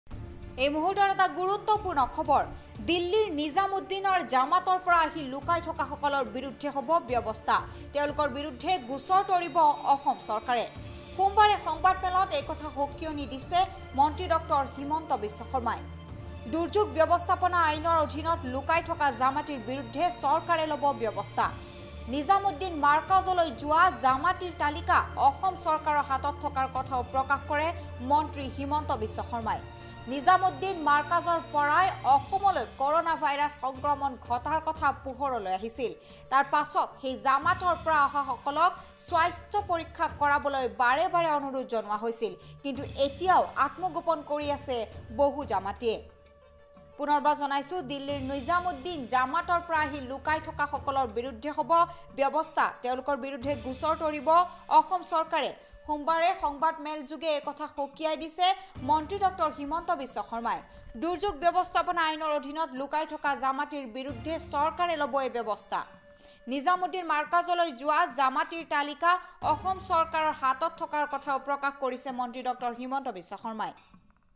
আত্মগোপন কৰি থকা জামাতীলৈ সকিয়নী মন্ত্ৰী ড০ হিমন্ত বিশ্ব শৰ্মাৰ
দিল্লীৰ নিজামুদ্দিনৰ জামাতৰ পৰা আহি লুকাই থকাসকলৰ বিৰুদ্ধে হ'ব ব্যৱস্থা
তেওঁলোকৰ বিৰুদ্ধে গোচৰ তৰিব অসম চৰকাৰে
সোমবাৰে সংবাদমেলত এই সকিয়নী দিছে মন্ত্ৰী ড০ হিমন্ত বিশ্ব শৰ্মাই
দুৰ্যোগ ব্যৱস্থাপনা আইনৰ অধীনত লুকাই থকা জামাতীৰ বিৰুদ্ধে চৰকাৰে ল'ব ব্যৱস্থা

নিজামুদ্দিন মাৰ্কাজলৈ যোৱা জামাতীৰ তালিকা অসম চৰকাৰৰ হাতত থকাৰ কথাও প্ৰকাশ কৰে মন্ত্ৰী শৰ্মাই